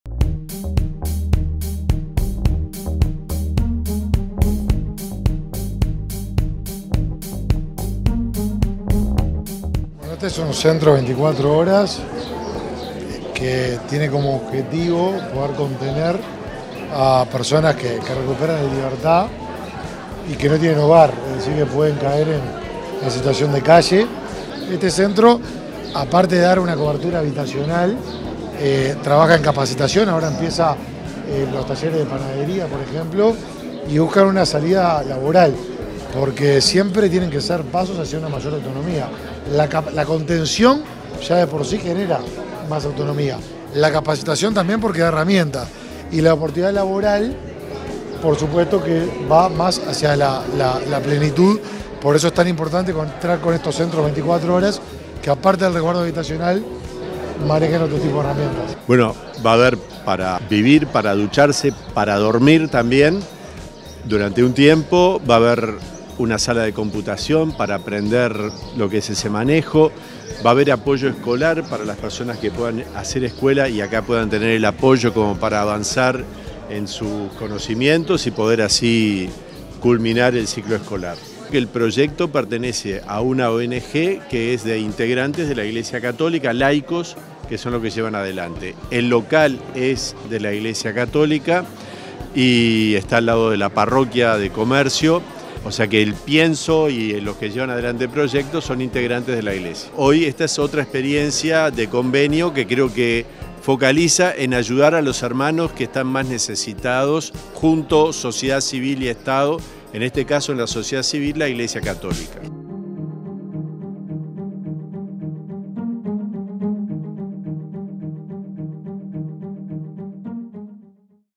ICM Noticias - Inauguración del Hogar Cardoner
En el corazón de La Unión y al lado de la parroquia Nuestra Señora del Carmen y San Cayetano, se encuentra el Hogar Cardoner, que el pasado martes 15 de agosto realizó su ceremonia de inauguración. Del evento, participaron autoridades eclesiales y del Ministerio de Desarrollo Social, además de los impulsores del centro de reinserción social.